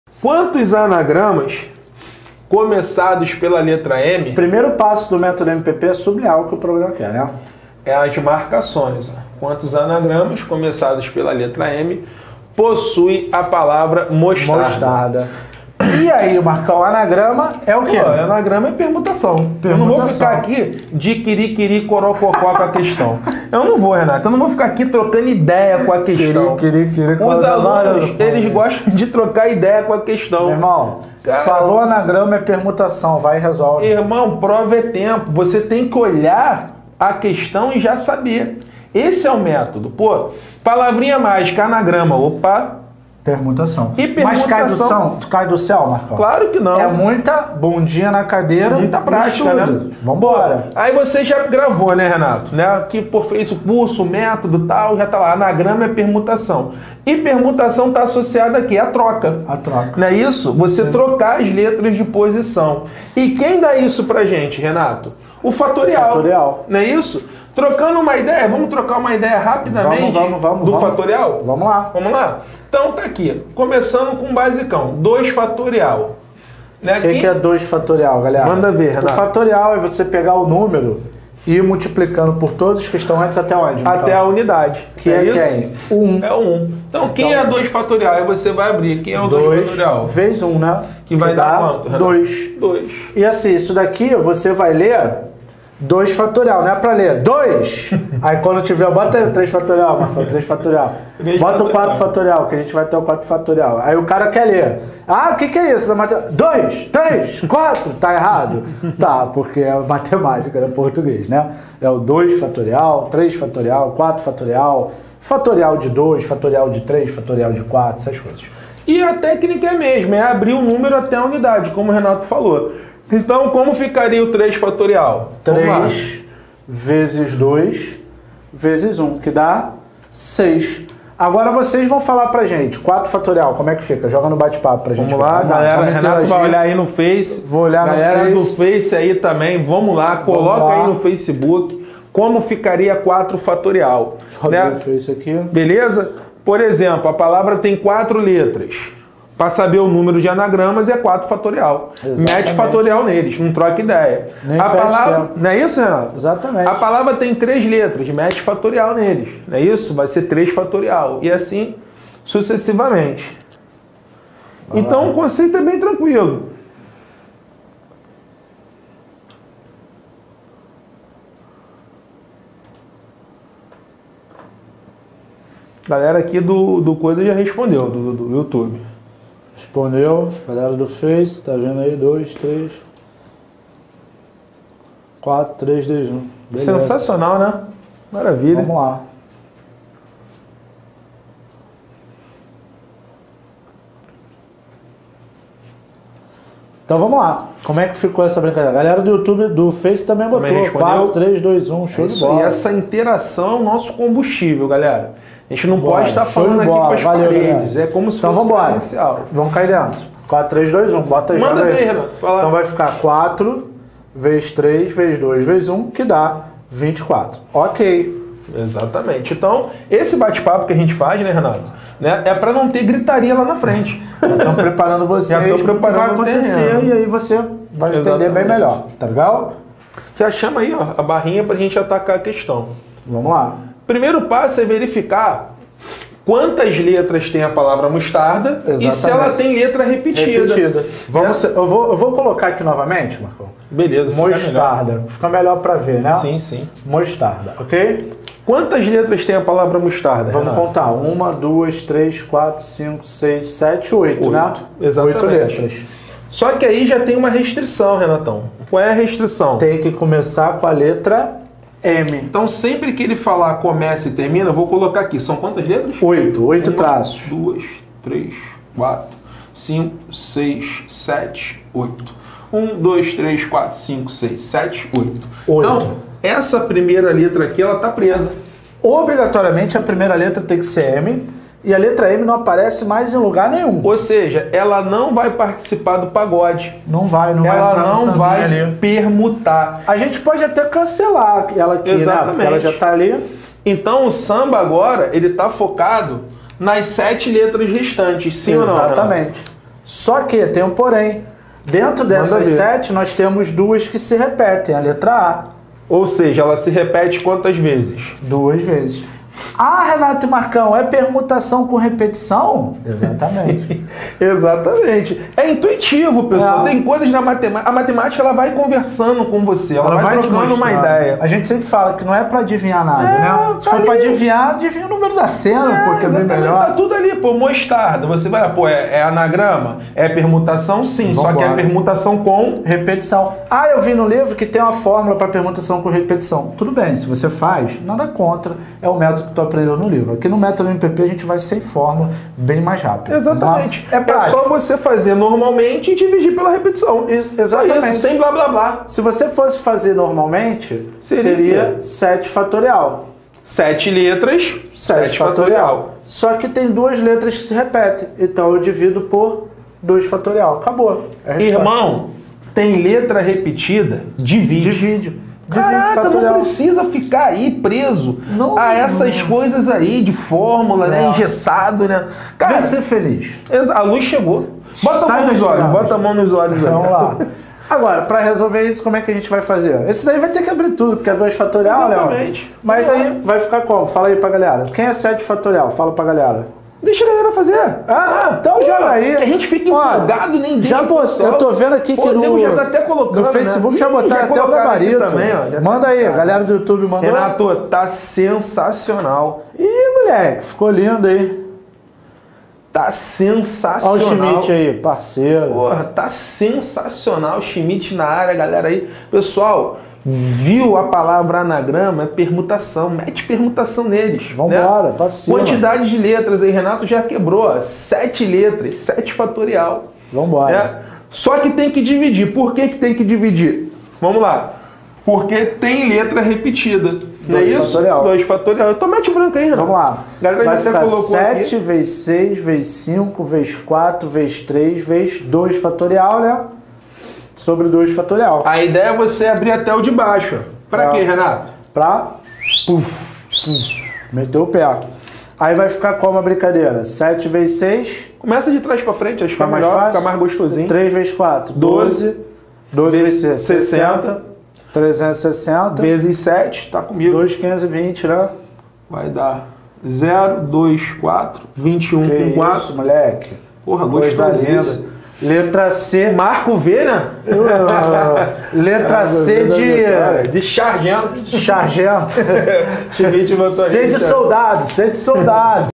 Matemática para PM PE |Dica 1| Anagramas, Você Sabe?
Então você precisa aprender com esse vídeo onde nosso professores , Renato e Marcão, mostra Matemática para PM PE. Para garantir muitos pontos na prova da Matemática e mandar muito bem na prova da PM PE